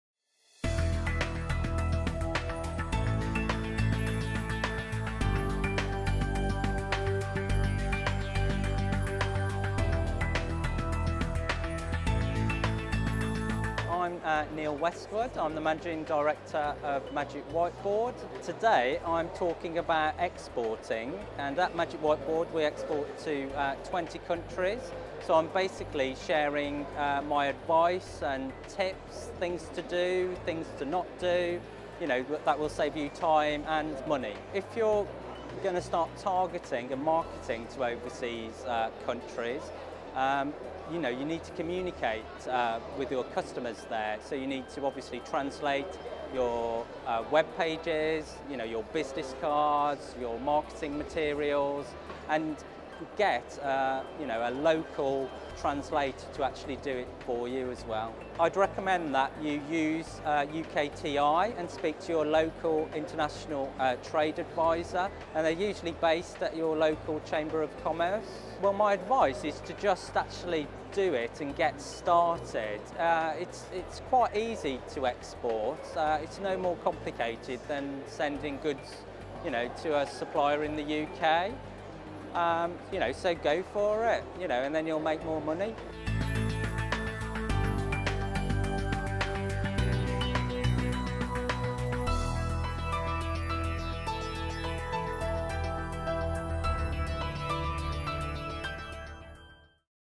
UKTI - Preview - From Dragon's Den winner to an international sensation
Neil Westwood, Managing Director of Magic Whiteboard shares his success story from Dragon's Den and beyond.

Key takeaways:
- Steps to get funding
- Getting the product known in international markets
- Translating your website for overseas customers